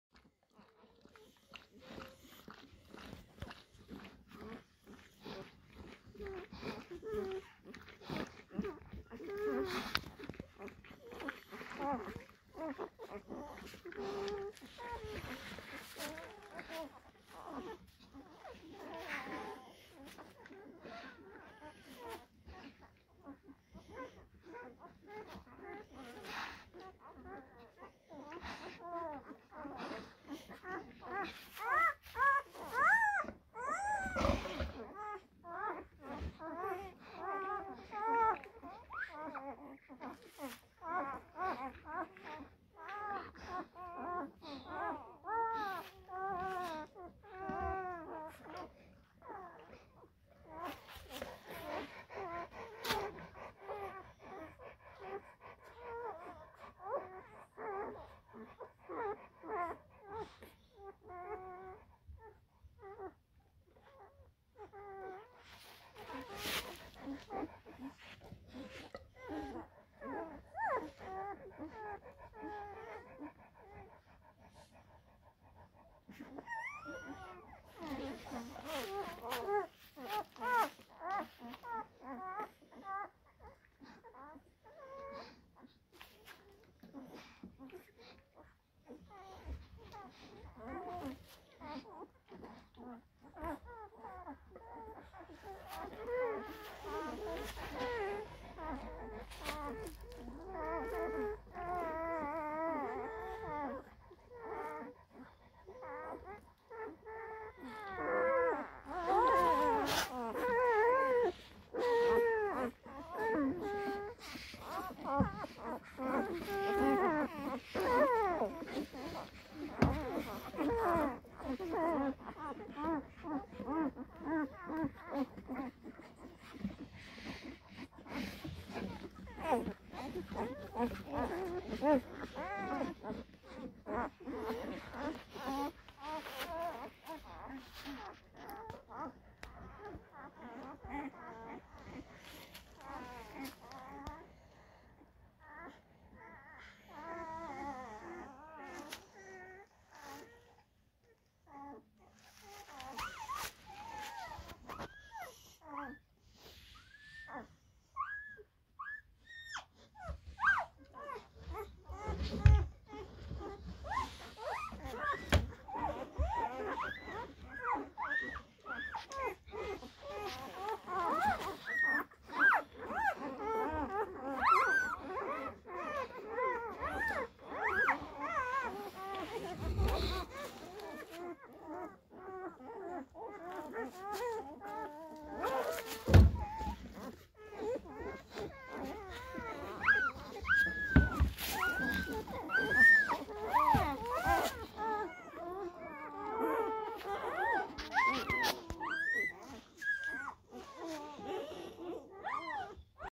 Alabai Welpen Mittelasiatischer Owtscharka Tag1
Tag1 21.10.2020  Unsere Hündin Hilda bekam 5 Rüden und 4 Hündinnen

Bei Interesse rufen Sie an: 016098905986